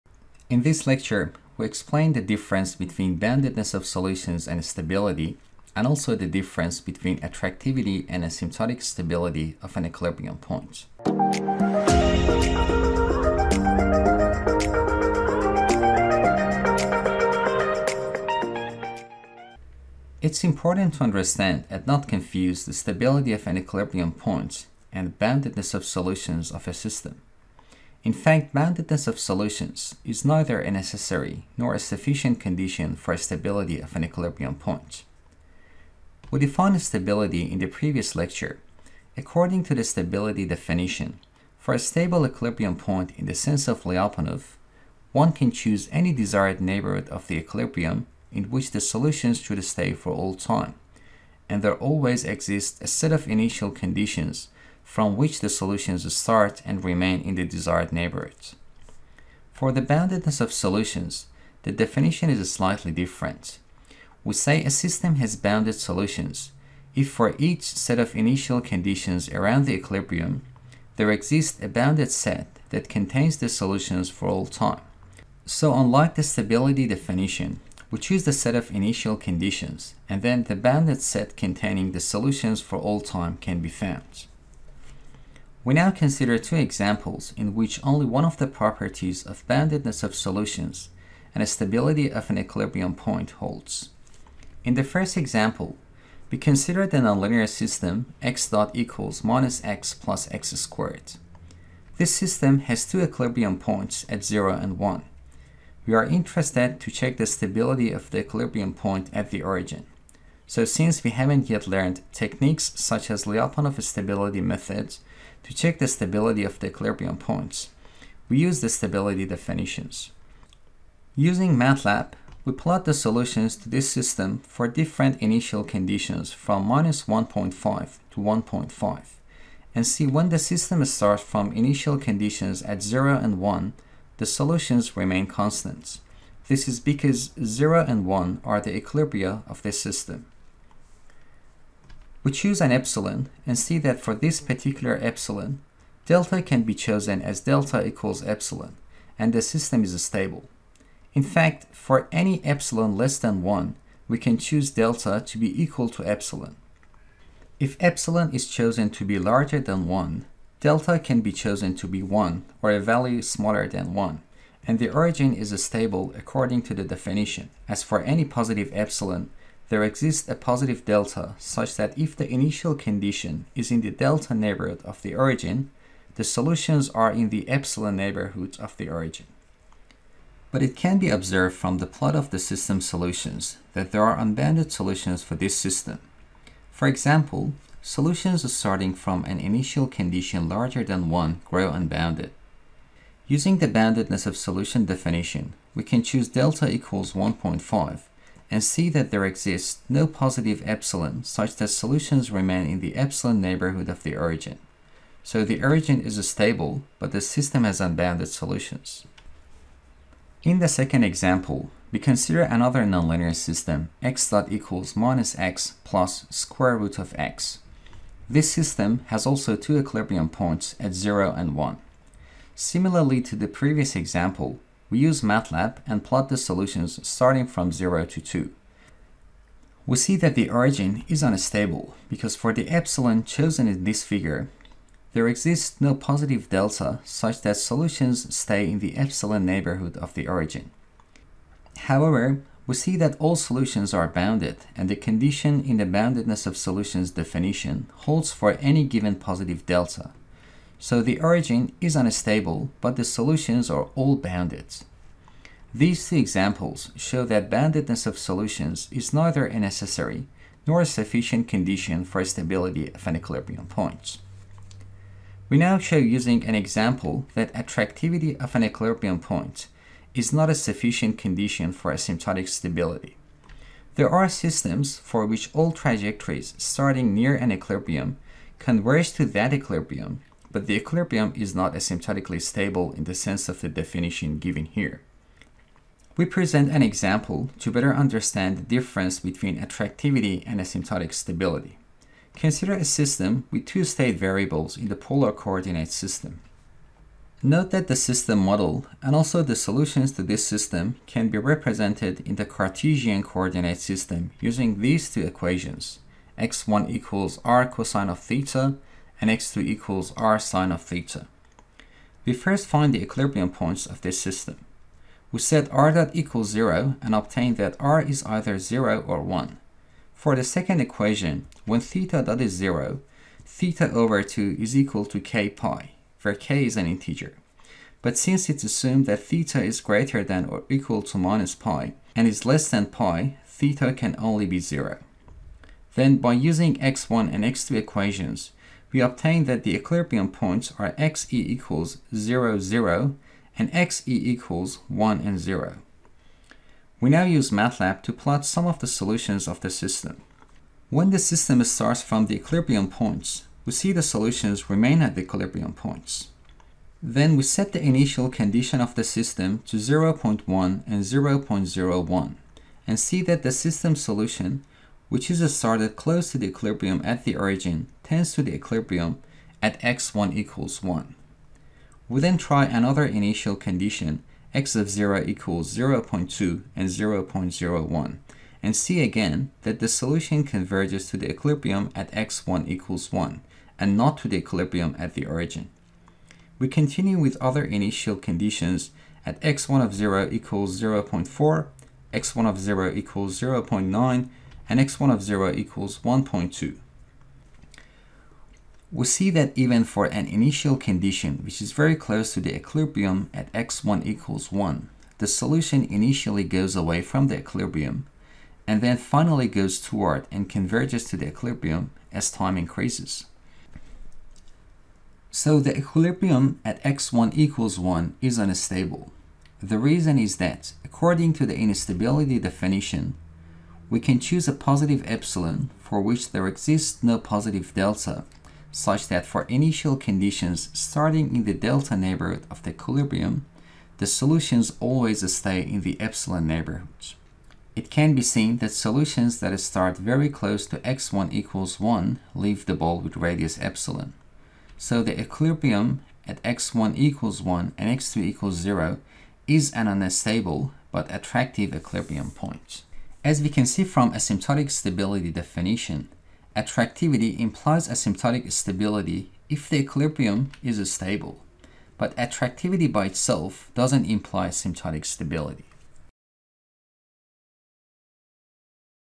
Nonlinear control systems - 1.4. Stability definition - Part 2
Lecture 1.4: Stability definition - Part 2

0:00  Introduction
0:20  Stability versus boundedness of solutions
4:26  Asymptotic stability versus attractivity of an equilibrium point